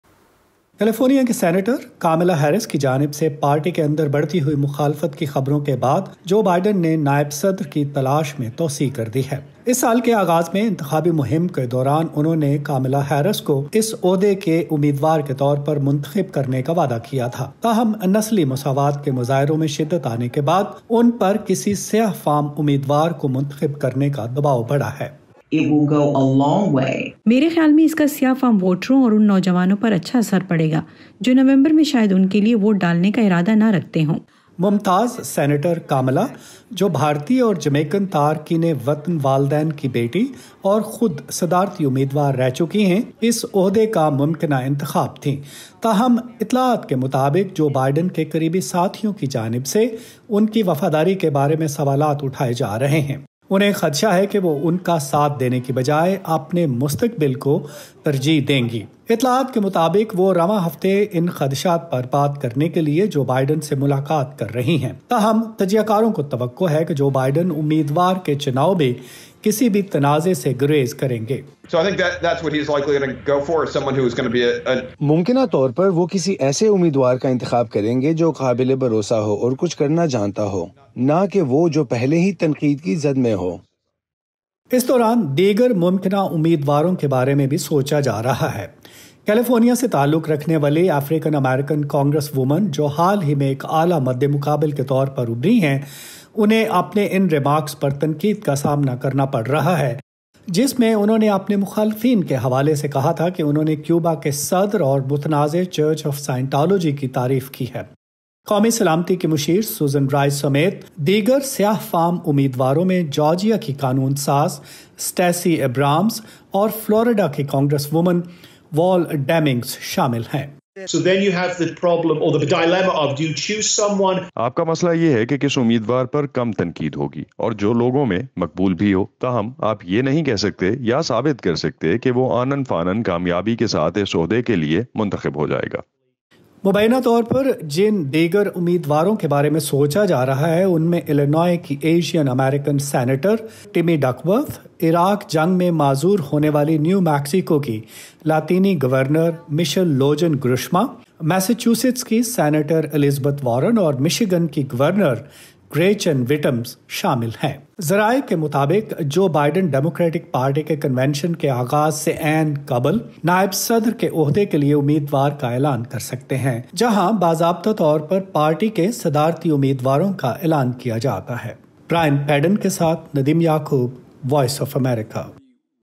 Who Will be Vice President Candidate from Democratic Party in US Elections?
اس سال کے امریکی صدارتی انتخابات میں ڈیموکریٹک پارٹی کی طرف سے نائب صدر کا امیدوار کون ہو گا؟ تفصیلات دیکھیے اس رپورٹ میں